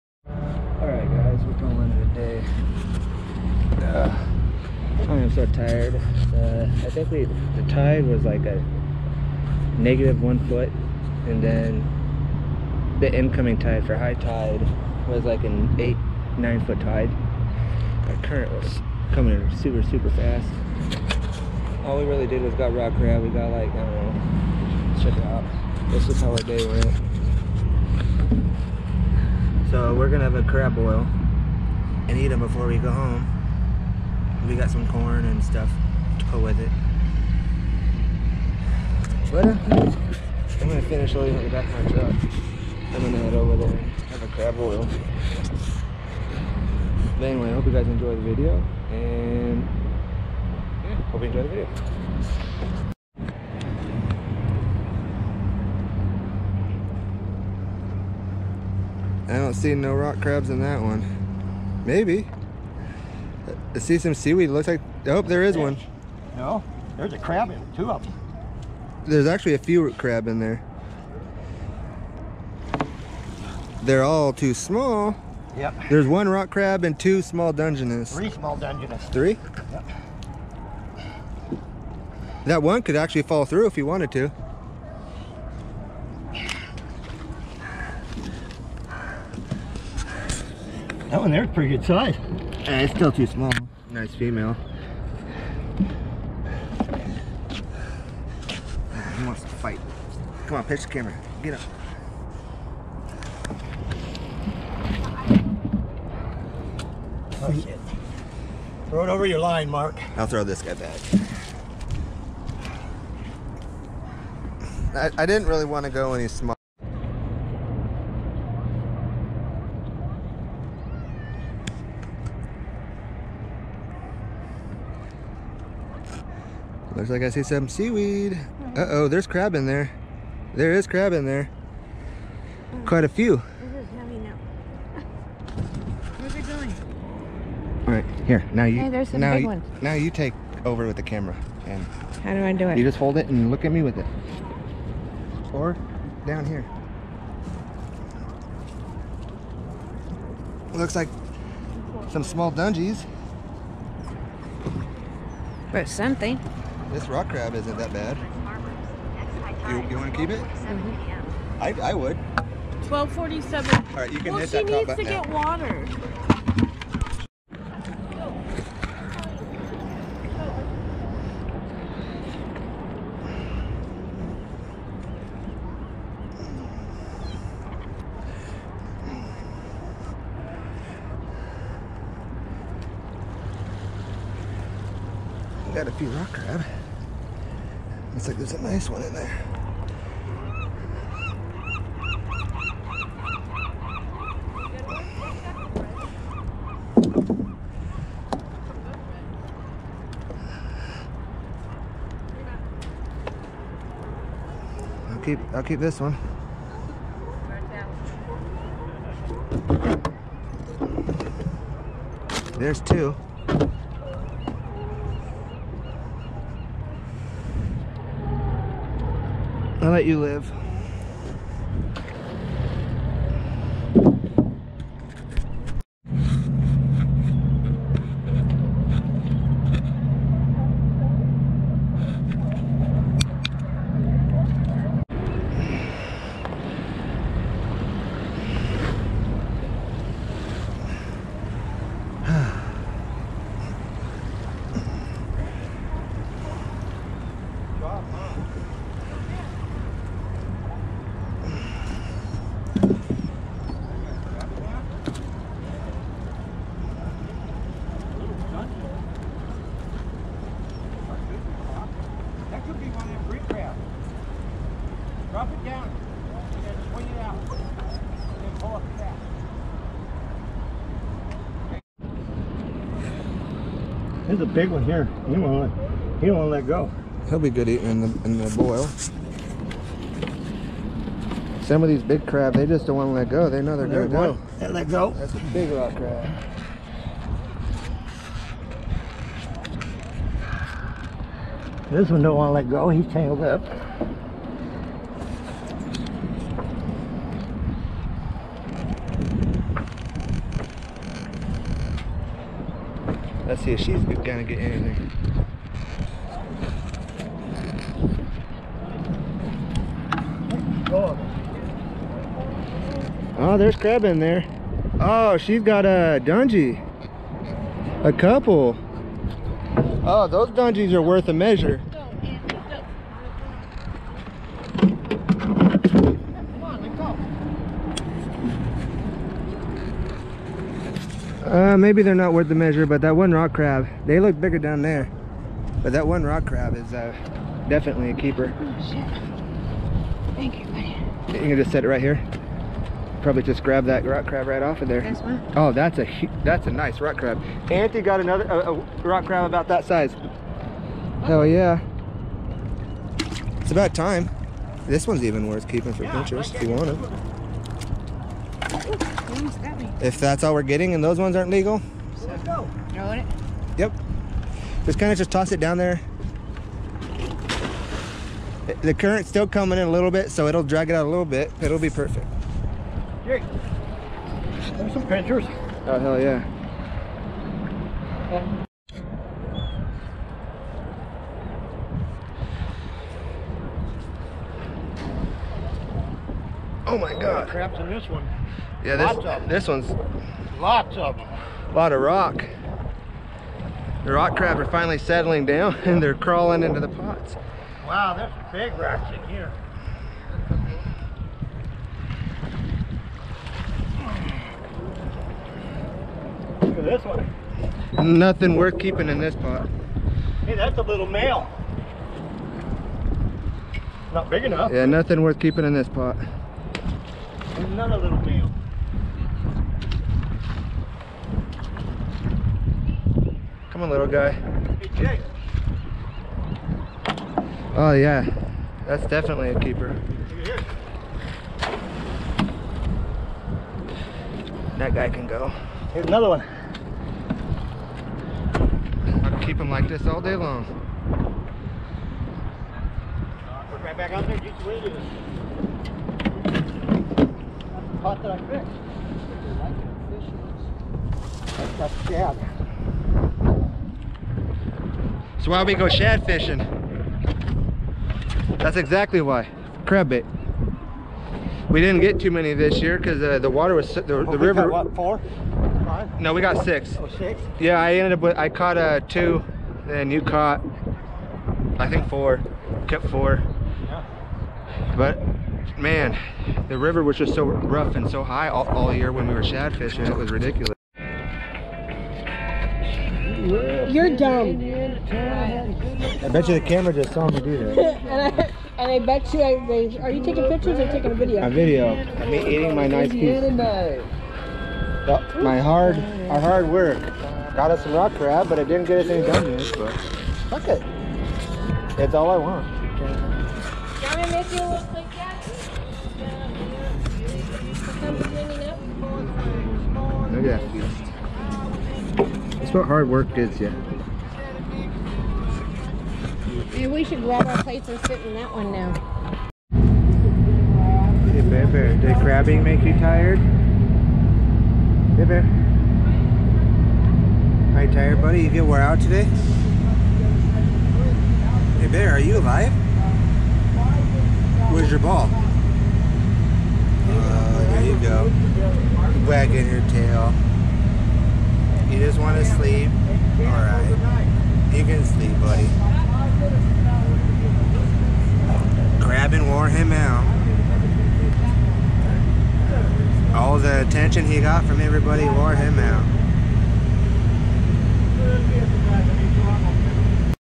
Crabbing at the Newport Historic Bayfront in Oregon
this was a fun day with the fam. we ended the day with a crab boil such good eats.